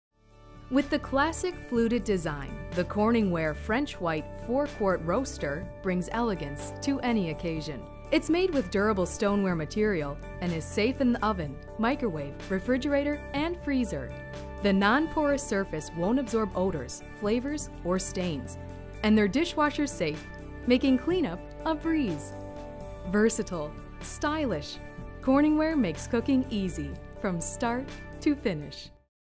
Corningware - 4 qt Roaster
This generous CorningWare® roaster is ready to hold a hearty ham, 10-lb. turkey, or robust roast. Plus veggies. Cooking's a snap: fill the roaster, place the lid on top, then put it in the oven. Sit back and await the compliments.